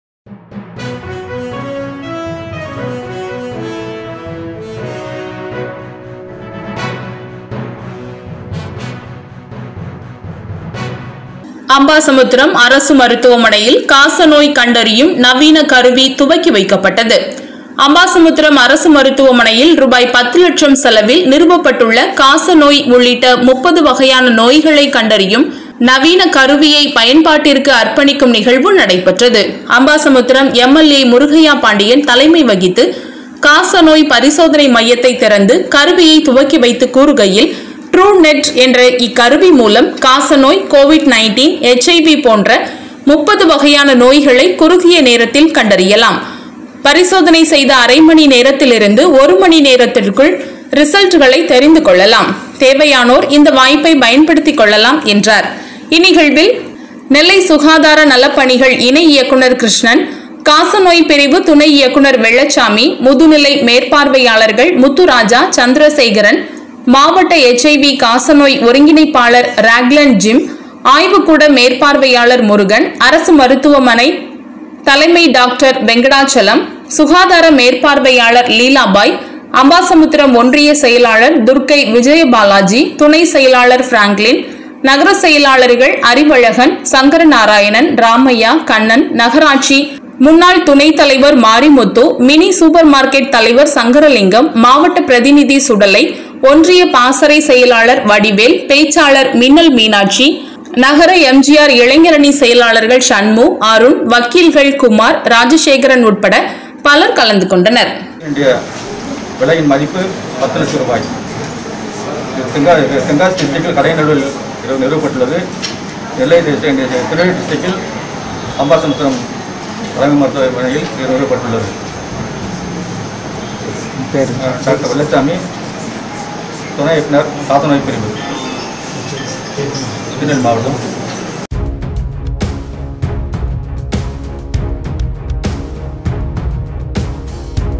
அம்பை அரசு மருத்துவமனையில் காச நோய் கண்டறியும் நவீன கருவி துவக்கிவைக்கப்பட்டது
AMBASAMUDRAM GOVERNMENTHOSPITAL அரசு மருத்துவமனையில் காச நோய் கண்டறியும் நவீன கருவி துவக்கிவைக்கப்பட்டது அம்பை MLA முருகையபாண்டியன் கலந்து கொண்டு பரிசோதனை மையத்தை திறந்து வைத்து கருவியை துவக்கி வைத்தார்